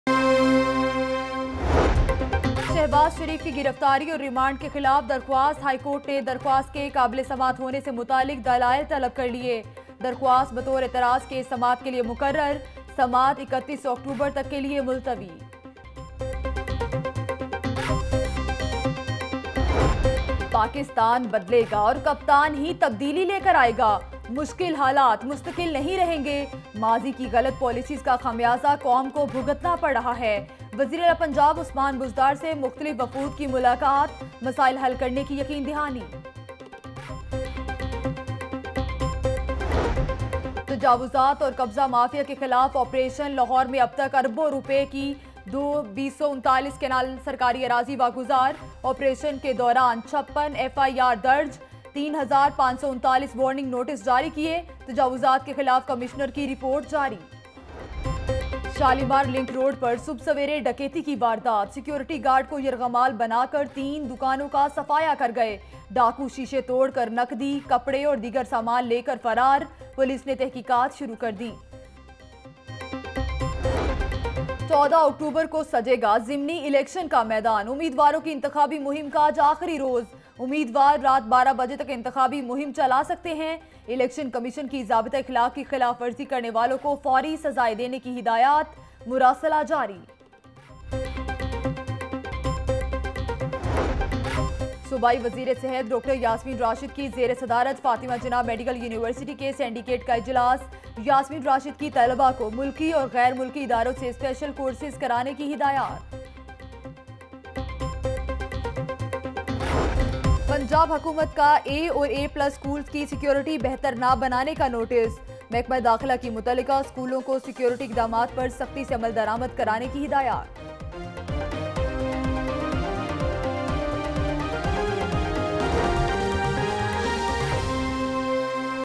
11 AM Headlines Lahore News HD – 12 October 2018
Watch Latest News:
Dollar price faces slight low, NAB arrests Former VC Punjab University, NAB orders to seize Ali Imran’s property, IG Punjab Muhammad Tahir replaced, PTI Govt appoints New IG Punjab, Shahbaz Sharif handed over to NAB on 10-days physical remand, Nawaz Sharif leaves for Islamabad to  appear before NAB Court in strict security, Hamza Shahbaz addresses PML-N workers convention, Large faction of workers pays tribute to Shahbaz Sharif, Uzma Bukhari challenges NAB, Maryam Aurangzeb press conference outside NAB Court, Model Town massacre SC forms JIT, Scuffle breaks out between locals and traffic wardens, Dollar prices witness sharp increase, Oil tanker collides at Waris Meer underpass and much more.